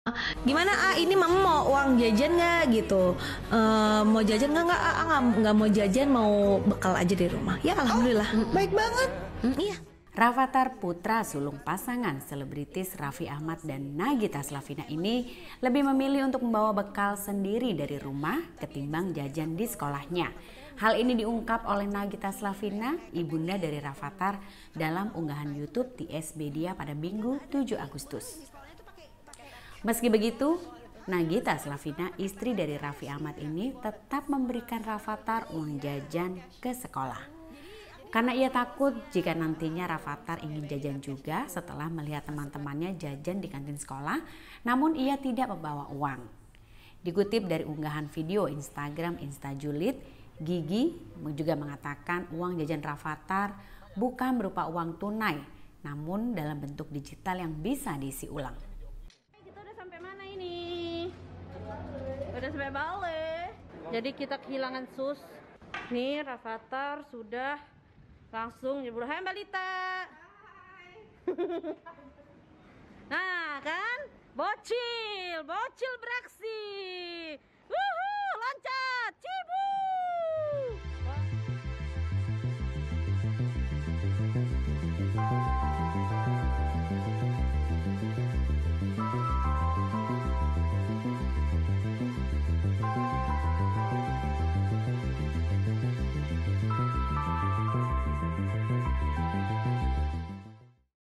Rafathar Putra Raffi Ahmad dan Nagita Slavina Lebih Pilih Bawa Bekal Daripada Jajan
WARTAKOTALIVE.COM, JAKARTA - Rafathar putra sulung pasangan selebritis Raffi Ahmad dan Nagita Slavina ini lebih memilih untuk membawa bekal sendiri dari rumah ketimbang jajan di sekolahnya.

Hal ini diungkapkan oleh Nagita Slaviana ibunda dari Rafathar dalam unggahan youtube TS Media pada Minggu (7/08/2022).

" Gimana A, ini mau uang jajan enggak? Enggak, Aa enggak mau jajan. Mau bekal aja dari rumah ya alhamdulillah" kata Nagita Slavina.

Laporan: Joanita Ary
Sumber: Instagram @insta_julid
Editor Video: Krisna